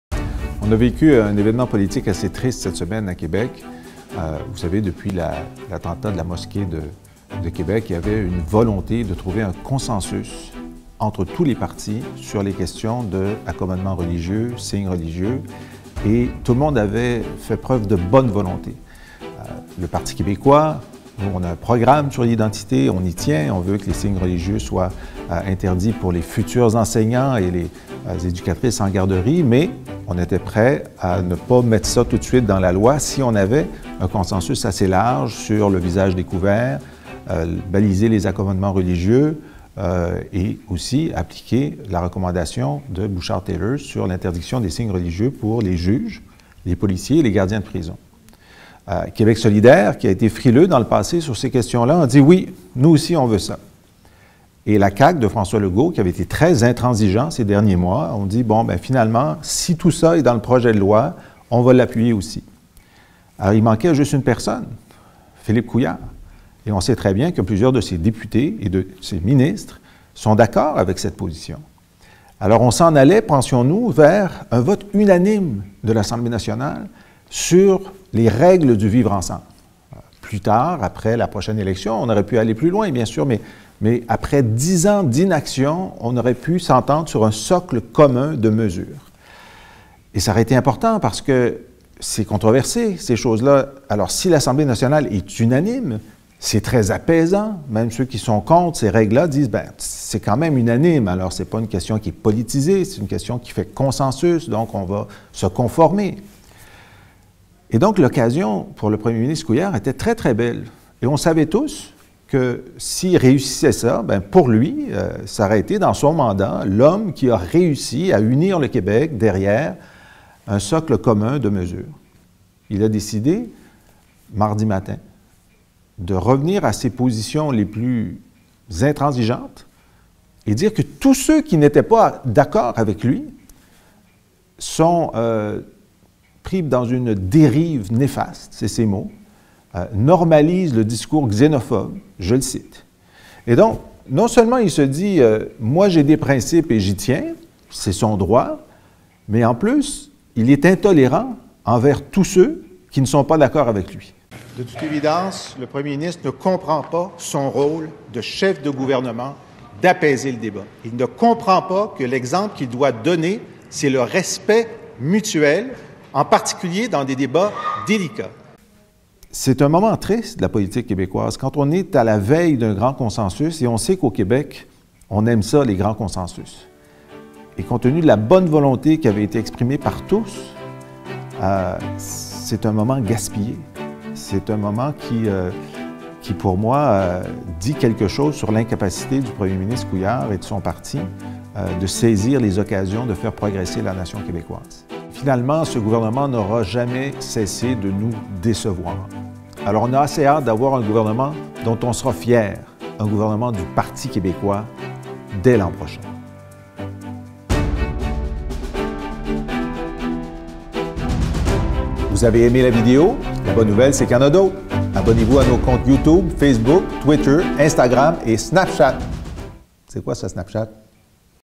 🙇🏻 ⛔️ Laïcité : par son intransigeance, Philippe Couillard rate l'occasion de faire l'unanimité
Jean-François Lisée fait le point sur le refus du premier ministre Philippe Couillard de faire consensus pour la laïcité au Québec.